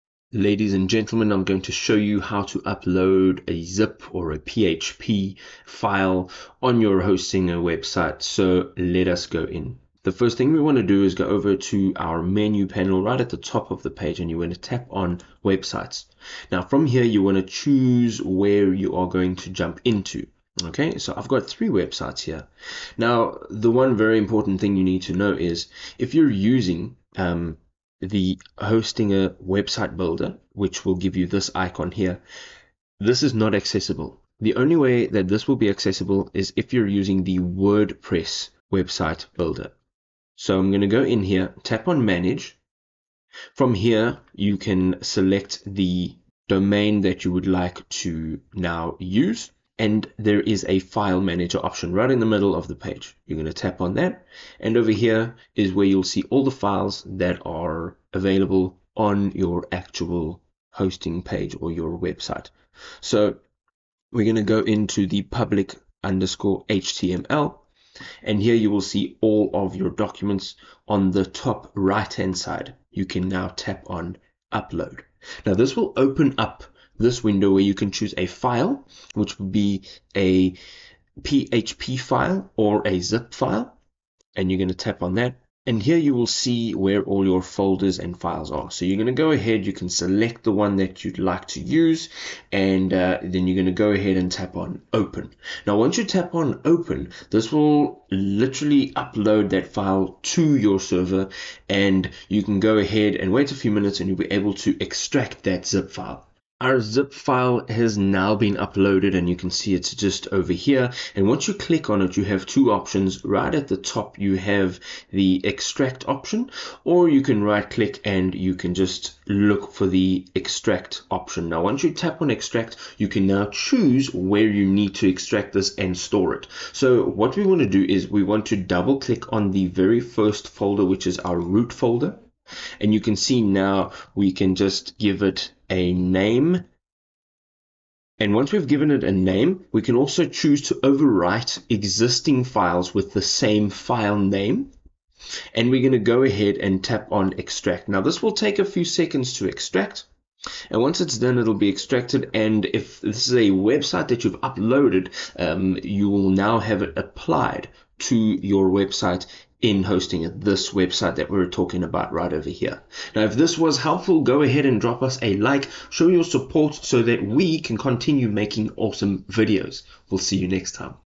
How to Upload Zip/PHP File in Hostinger | Hostinger Tutorial 2025
In this tutorial 'How to Upload Zip/Php File in Hostinger | Hostinger Tutorial', we will guide you through the process of uploading a zip or PHP file to your Hostinger hosting account. Uploading a zip or PHP file is a common task when you want to install a new application, upload a backup, or transfer files to your hosting account. By following the steps outlined in this tutorial, you can easily upload a zip or PHP file to Hostinger, ensuring that your files are securely transferred and ready for use. Stay tuned as we walk you through the process of uploading a zip or PHP file in Hostinger, providing you with the necessary knowledge to manage your files efficiently and maintain a reliable hosting environment.

Timestamps

Intro – 00:00 – 00:09
Uploading Zip/Php File in Hostinger – 00:10 – 02:51